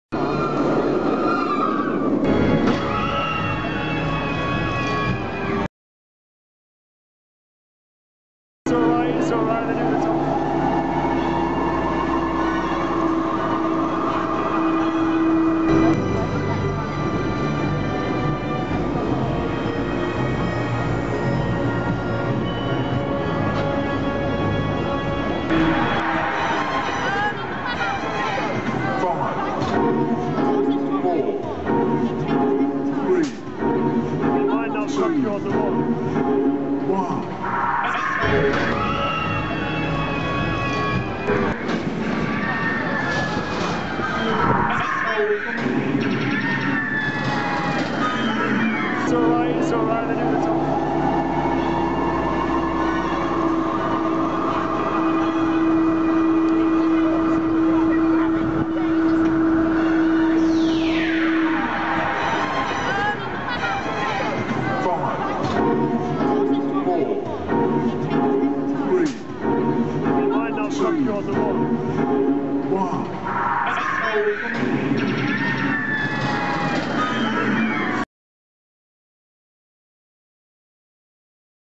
Detonator - Thorpe Park - Offride HD
Detonator is Thorpe Park's Fabbri Drop Tower, standing at 115 feet in the air! This flat ride achieves a speed of around 50 mph. Watch this from various offride perspectives.